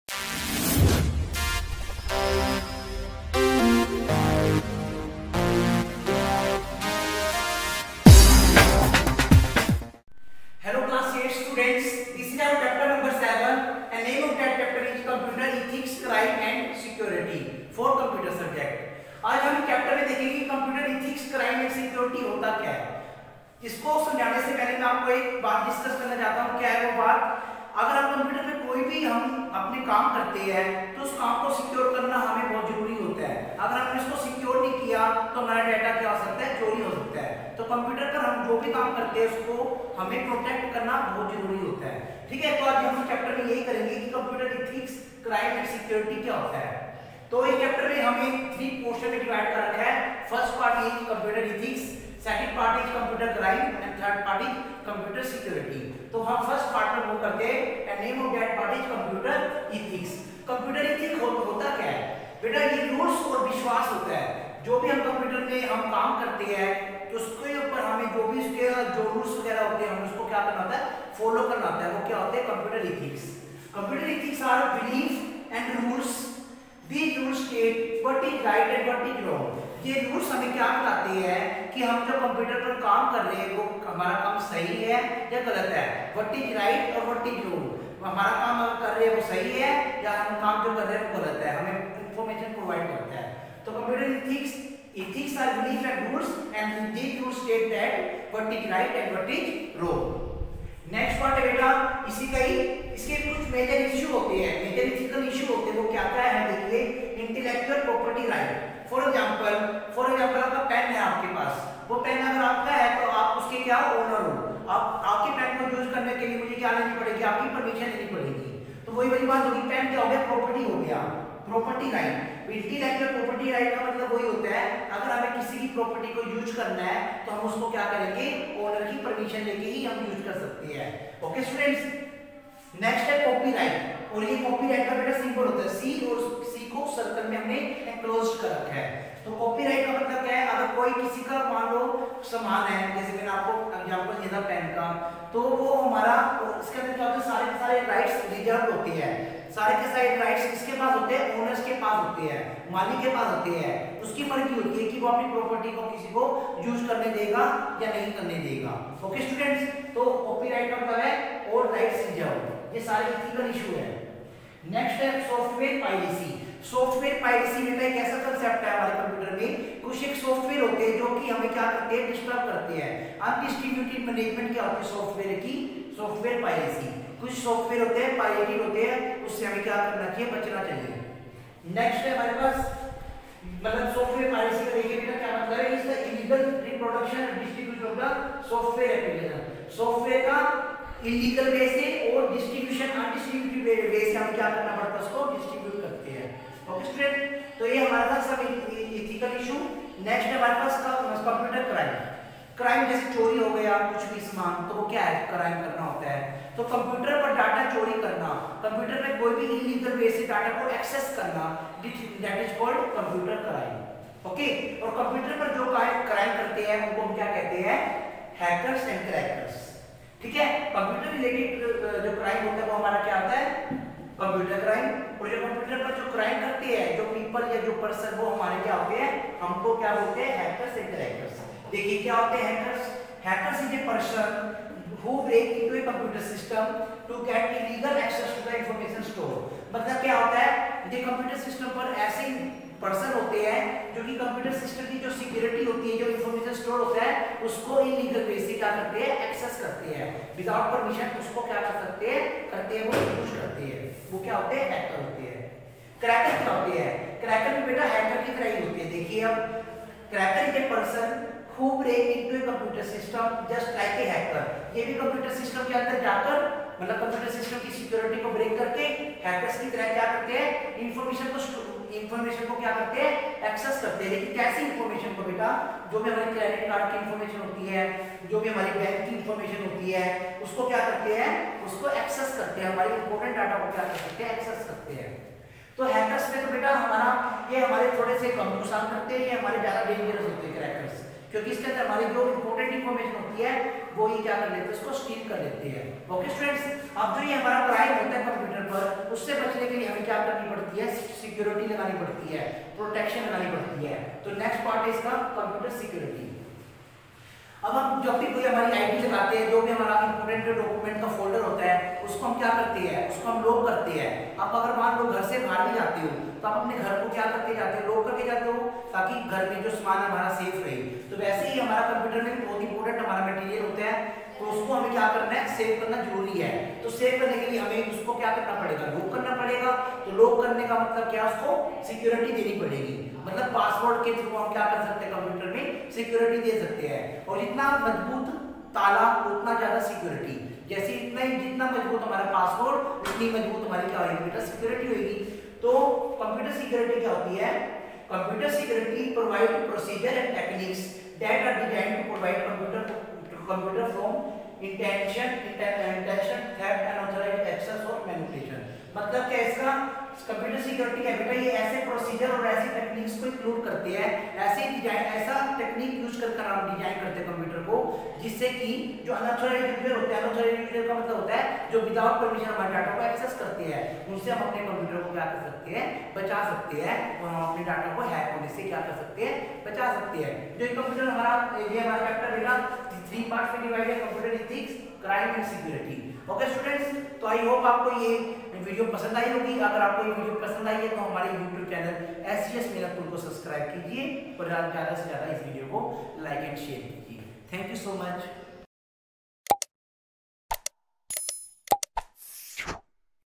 Computer Ethics, Crime and Security # Ch-7 # COMPUTER # 8th standard # SGS Milakpur # By KG sir
Dear Students
This is official you tube channel of Sidhivinayak Global School, Milakpur. Find here educational videos.

In this video, Computer Ethics, Crime and Security ( Ch-7 ) of 8th standard COMPUTER has been explained. Keep learning Keep watching.